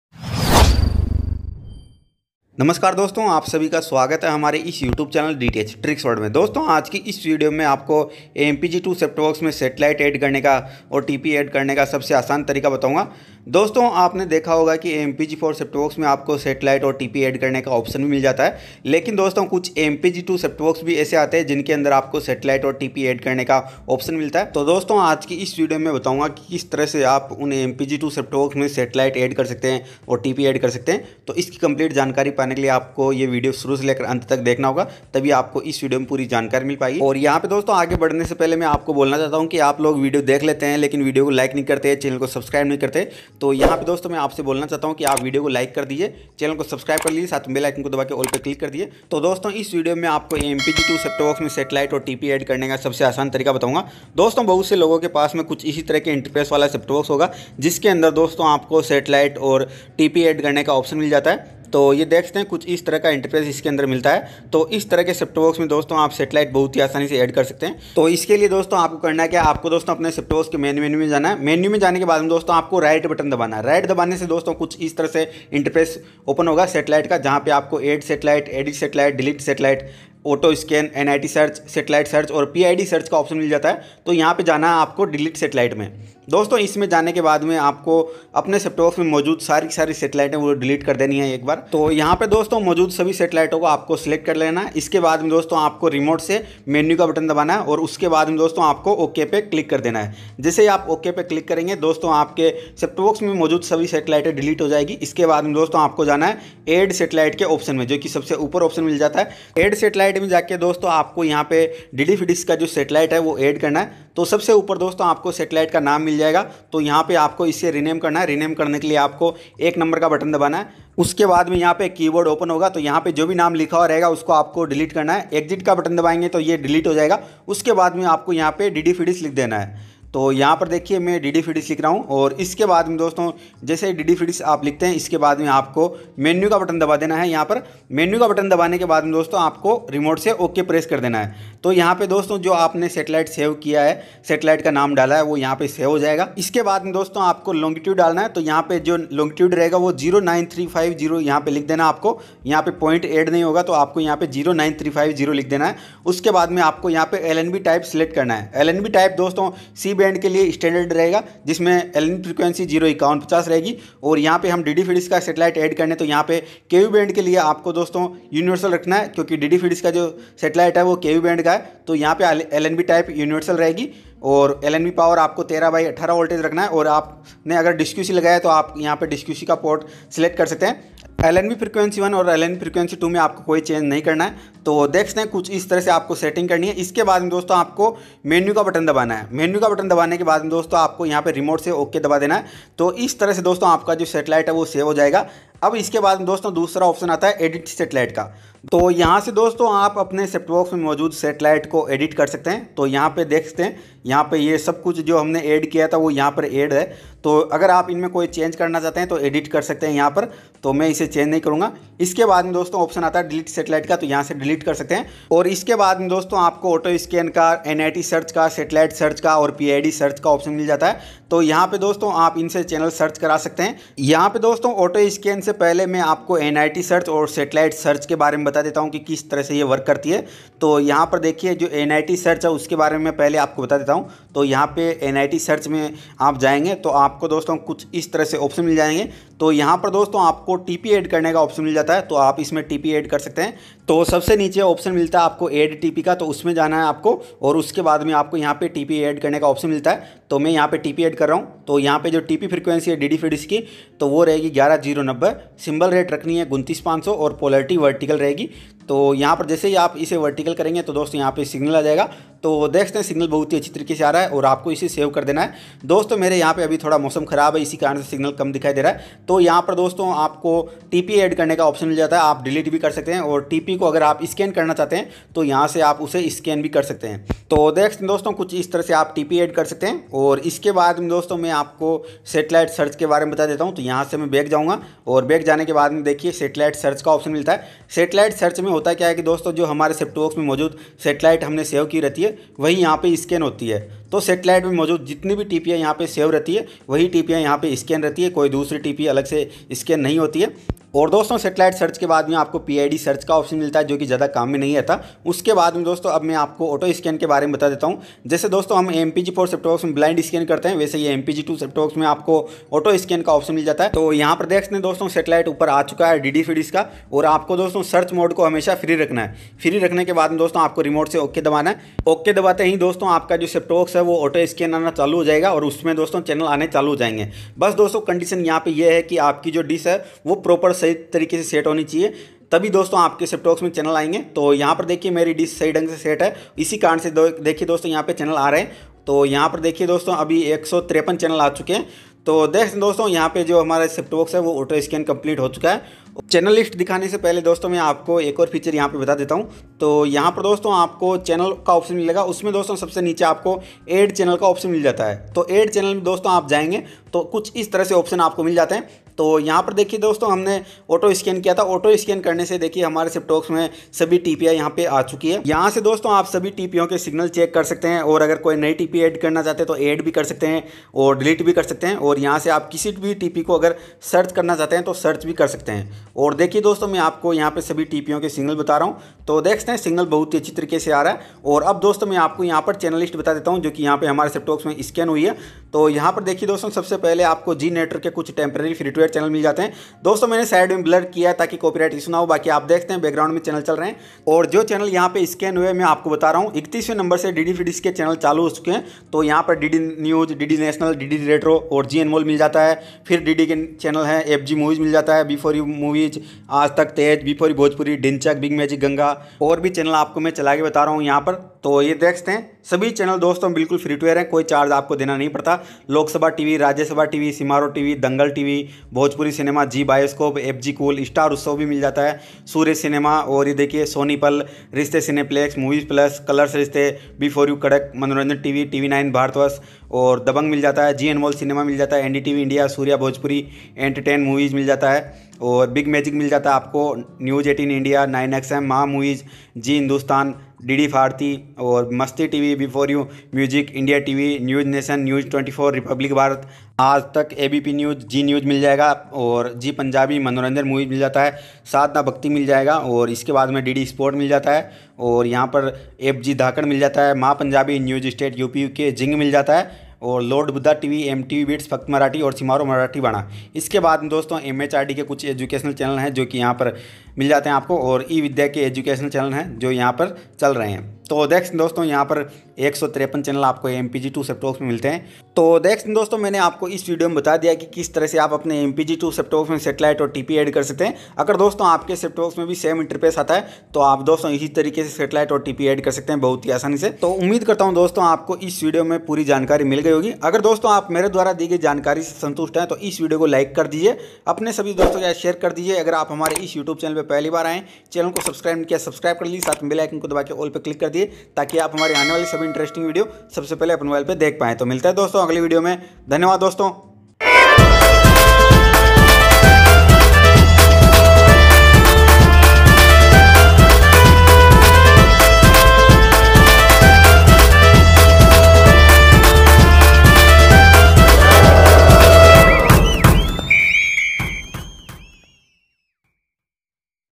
DD Free Dish MPEG-2 Set Top Box Settings | New Satellite और New TP Frequency Add करो MPEG-2 Box में
Doston is video me mai aapko DD Free Dish ke MPEG-2 Set Top Box me New Satellite aur New TP ko add karne ka sabse best tarika bataunga. To iske liye aapko ye video pura dekhna hoga.

✅Full HD MPEG-4 Set Top Box

✅MINI LED TV 4.5 Inch Display